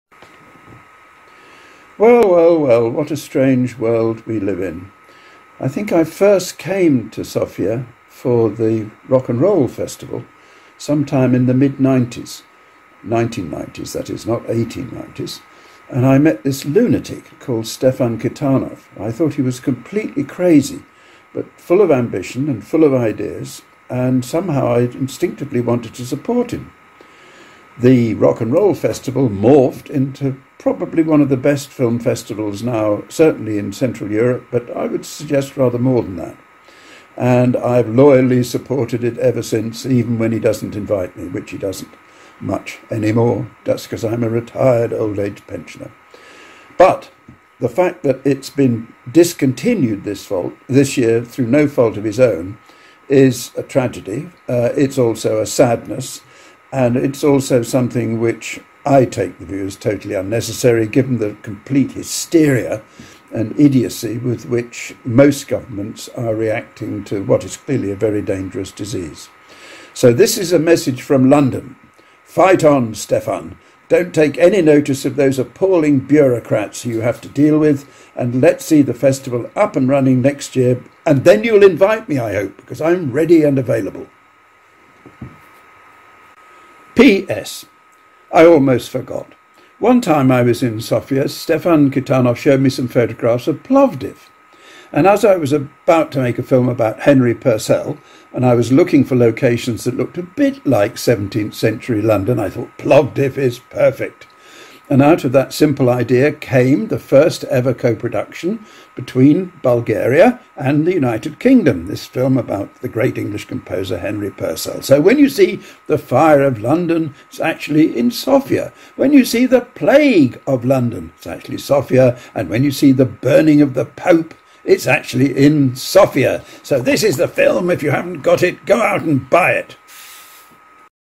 Tony Palmer * special Sofia Film Festival's friend *
"What a strange world we live in!"
British film director Tony Palmer, a very special friend of SIFF and Kita has a personal video message to all of us!